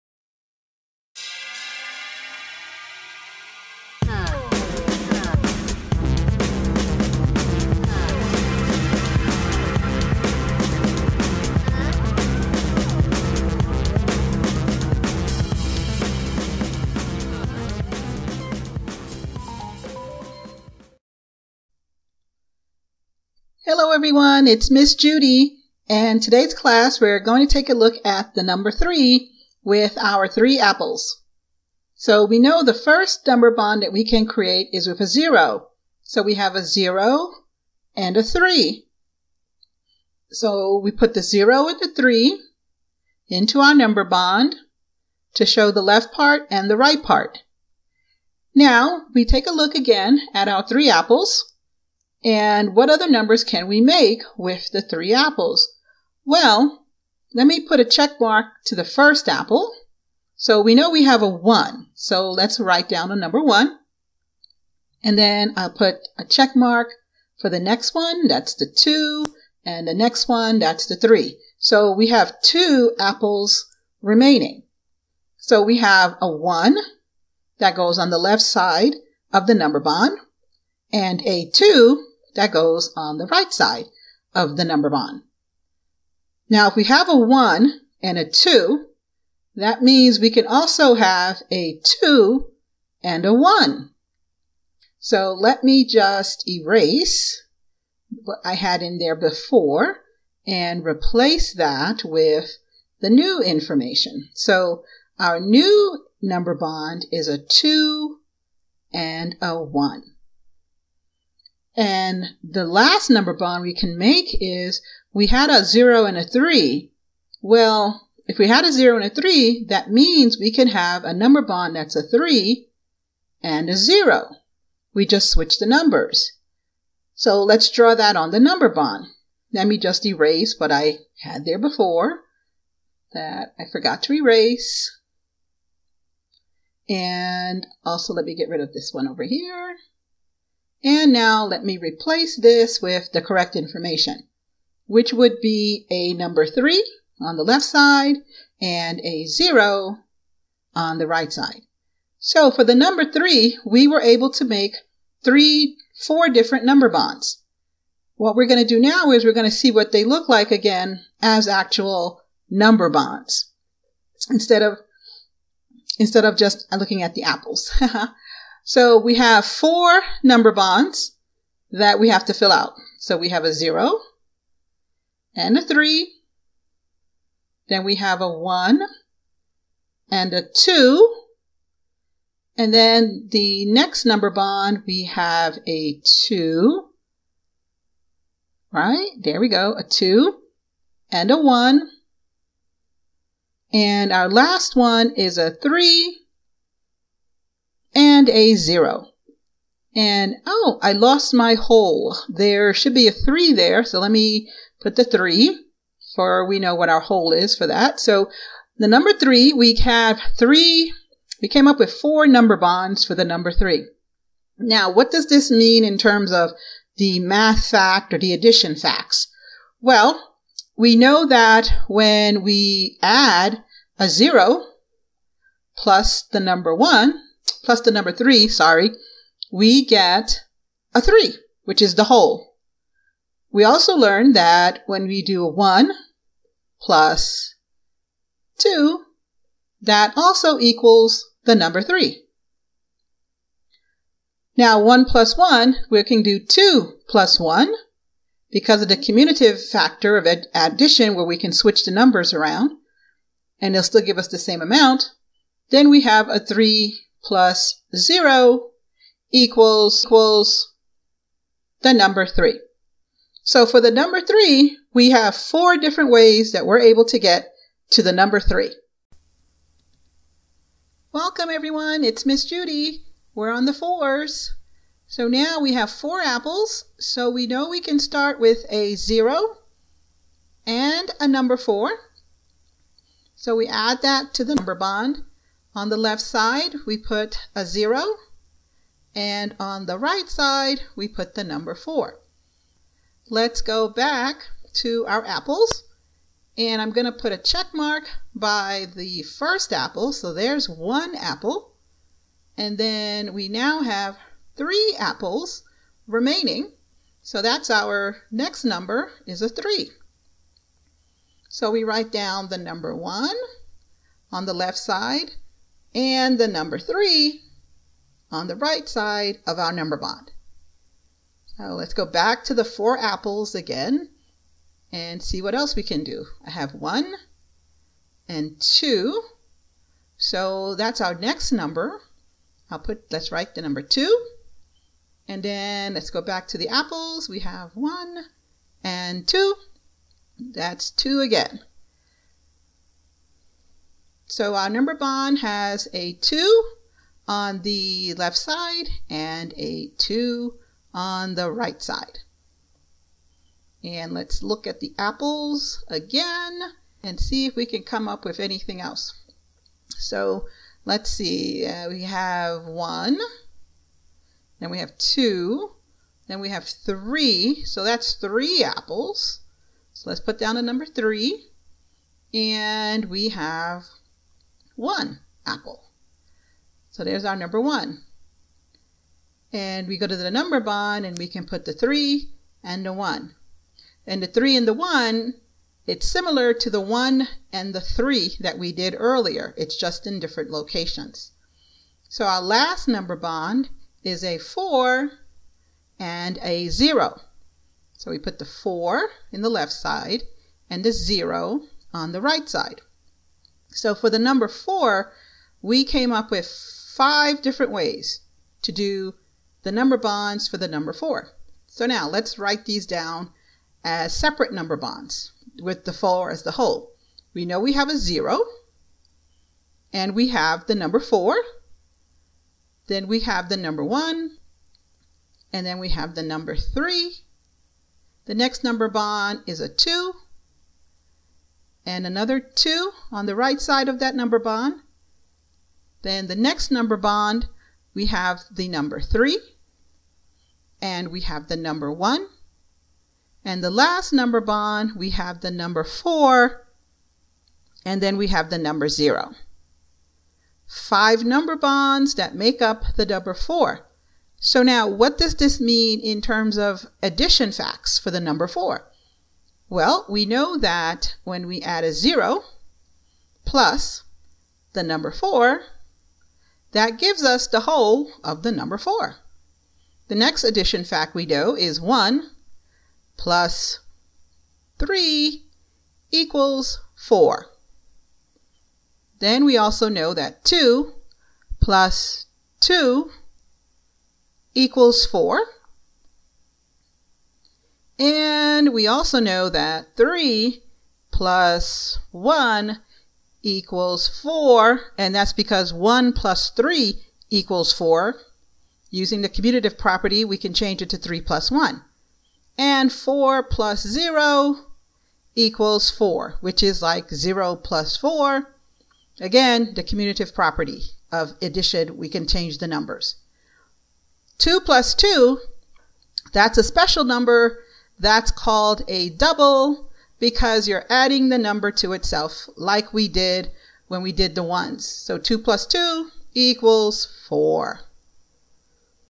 Number Bonds for 3 and 4
Learn how to make number bonds for 3 and 4 and how they can be used to make addition math facts.

Thanks!